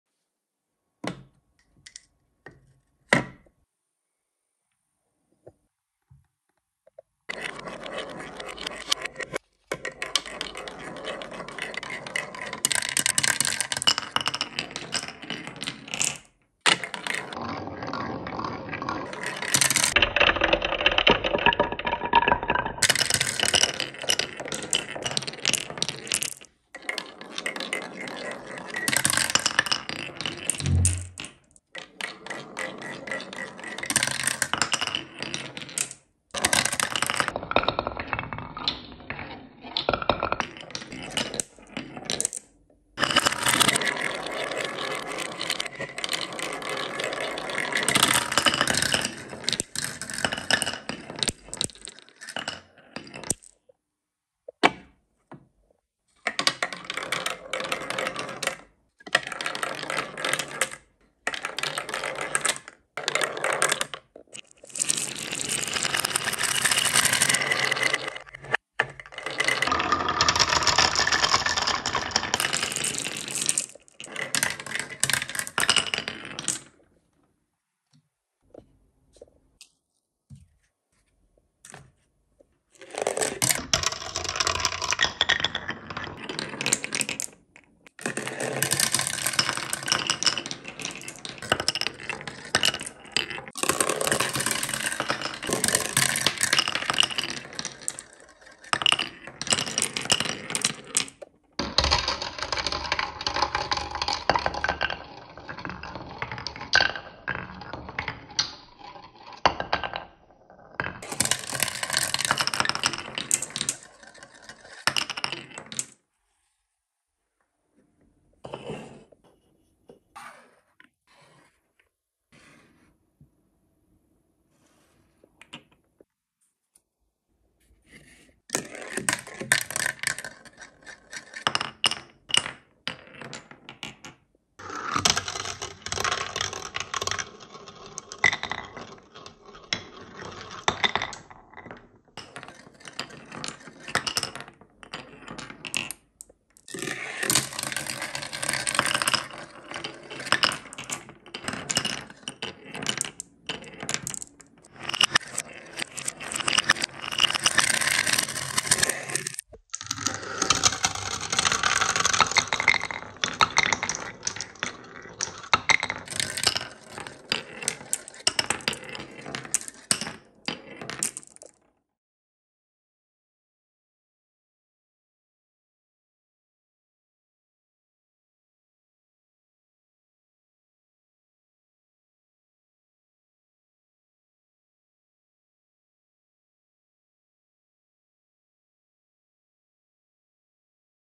Marble Run Race ☆ Double Wave Wooden Slope | マーブルランレース | Limitless Marble Race
▶️ A brand new content surprise awaits those who have a passion for watching marble run race videos. Get ready to experience the thrill and excitement with our double wave wooden slope, guaranteed to provide a sense of relaxation and freedom from stress. Prepare to be captivated by this visually stunning video, featuring vibrant marbles rolling through a specially crafted wooden track. Get ready to embark on an unforgettable marble run journey!

▶️ All content is exclusively owned and created by Limitless Marble Race